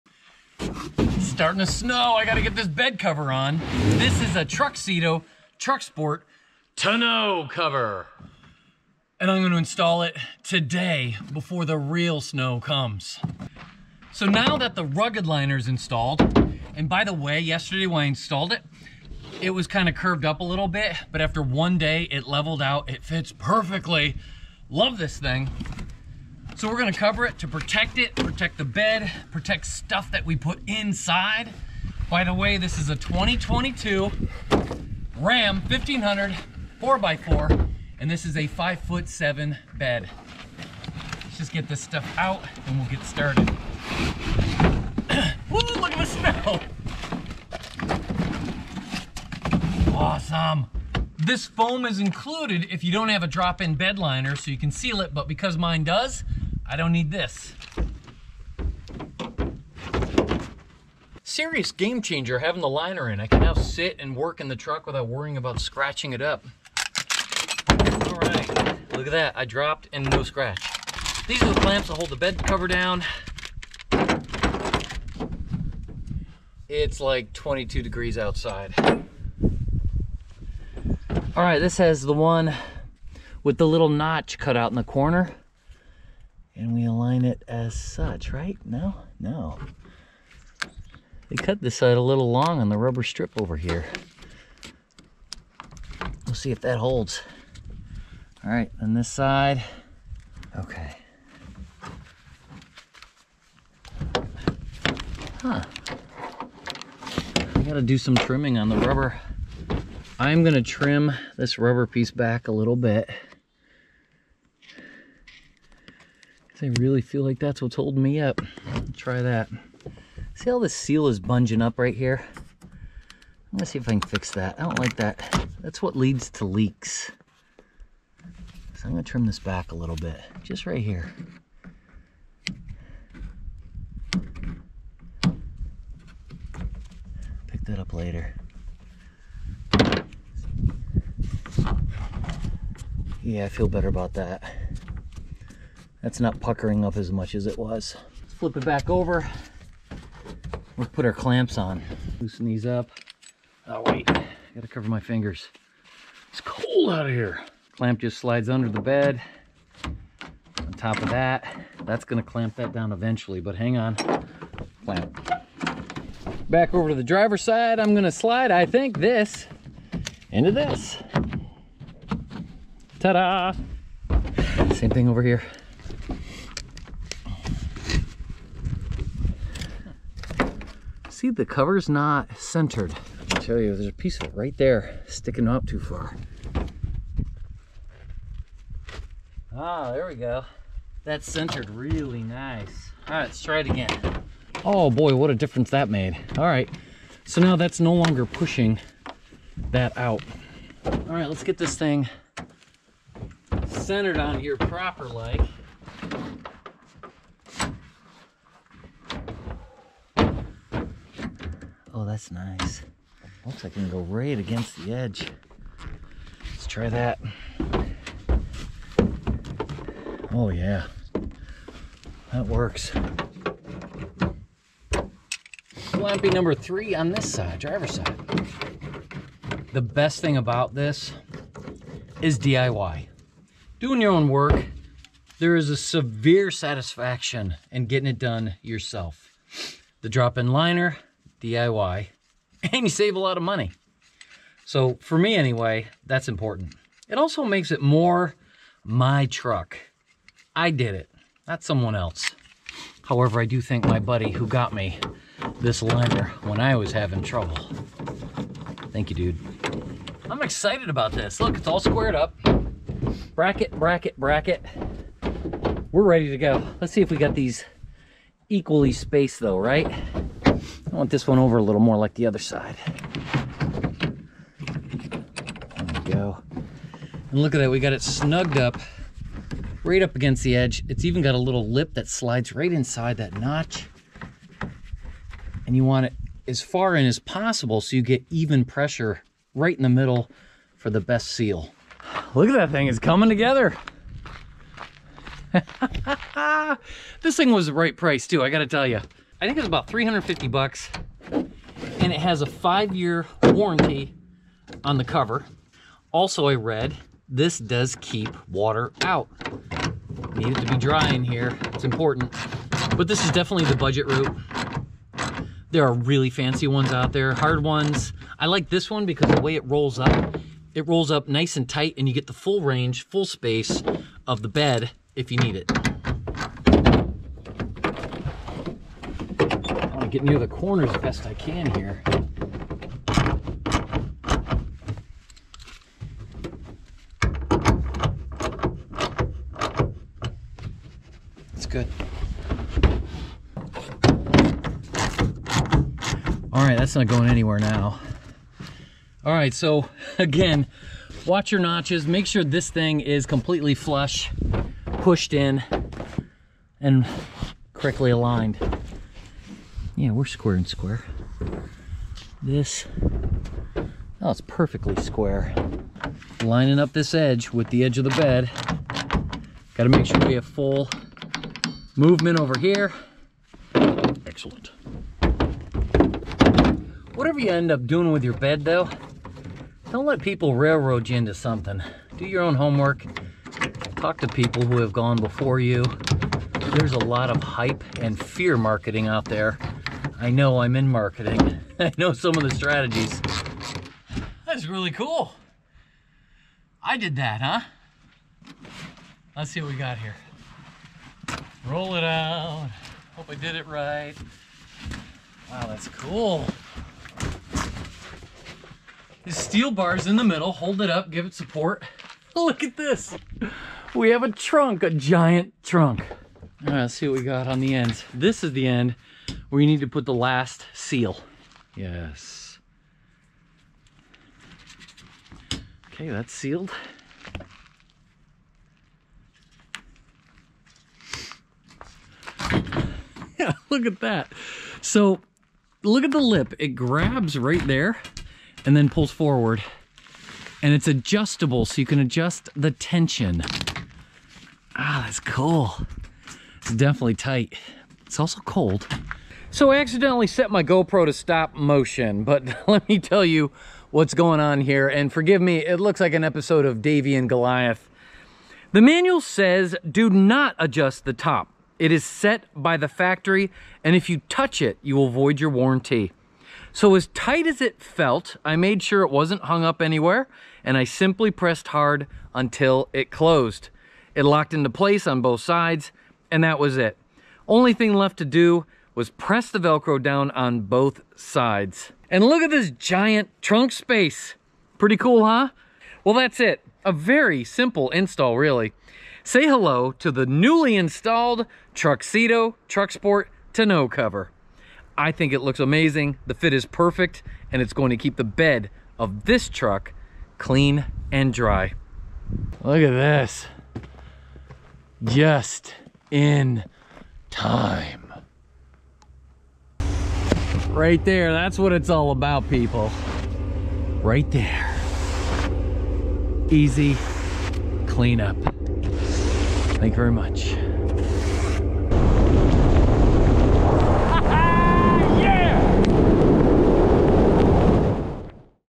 Upgrade Your Ram 1500 with a TruXedo TruXport Soft Roll-Up Truck Bed Tonneau Cover
Digging Vermont snow and ice out of the back of a pick up truck is NO FUN! So I ordered it and it arrived just in time as the snow flakes began to come down. This is the unboxing. Installation. And finished result. And I am so happy with it!